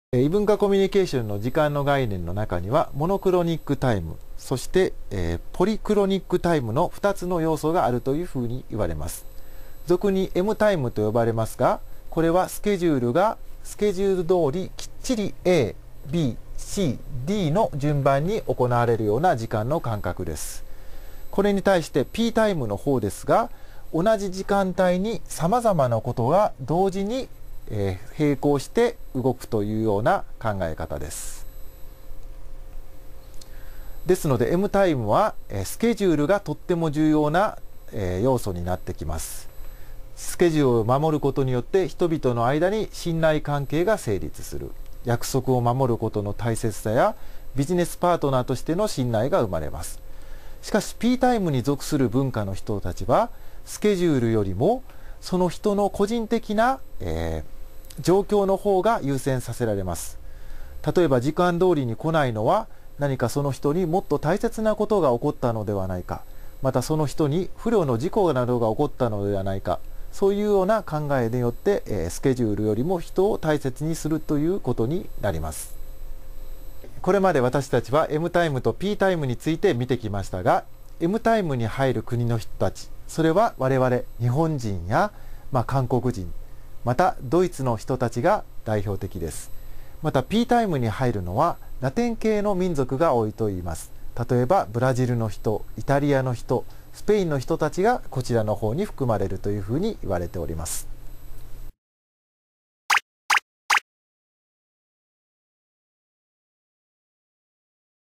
３分でわかる「異なる時間概念／MタイムとＰタイム」
「高校生のための『超』教養講座」オフィシャルサイト。

育った場所が異なる人と会話をした際、話が噛み合わない経験をしたことがある人は多いはず。同じ日本人でもそうなのですから、国や文化が違えば受け止め方はもっともっと異なります。同時に、この文化や習慣の壁が言葉以上に、円滑なコミュニケーションや相互理解にブレーキをかける要因となっています。これをどう乗り越えるか。文化間の違いを理論化し、正しいコミュニケーションの方法をわかりやすく解説する「高校生のための『超』教養講師＝東京外国語大　岡田昭人・准教授講座」の講義より、文化によって異なる時間感覚「Mタイム」と「Pタイム」に関する部分を切り取りました。